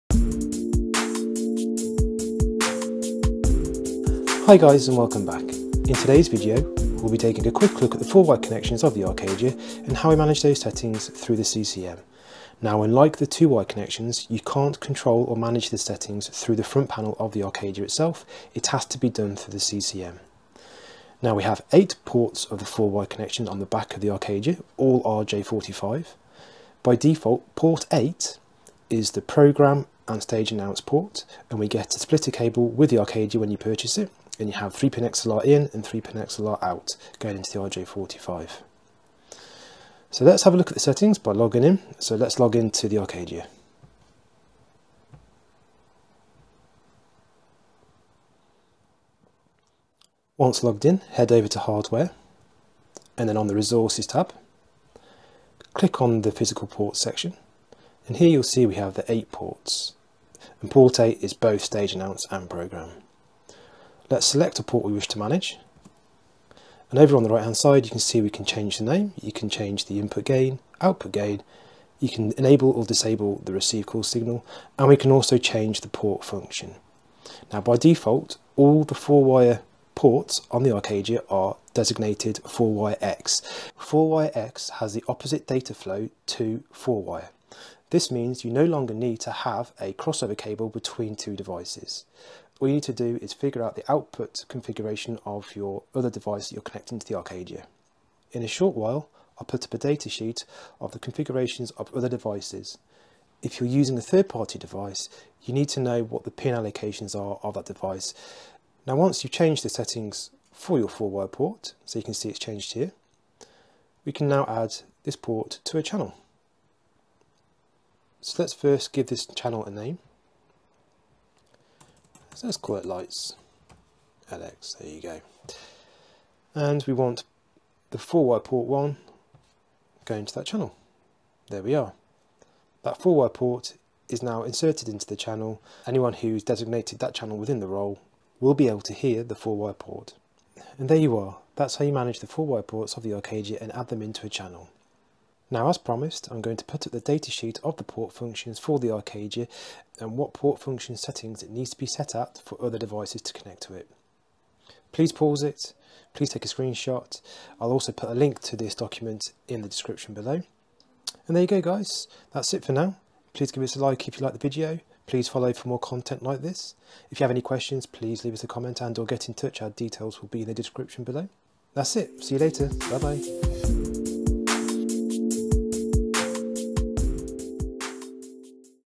Clear-Com Arcadia – Interfacing with 4-wire Systems
In this video Tyrell's Clear-Com expert will show you how to interface 4-wire systems with Clear-Com's Arcadia.

to discuss your intercom/talkback requirements.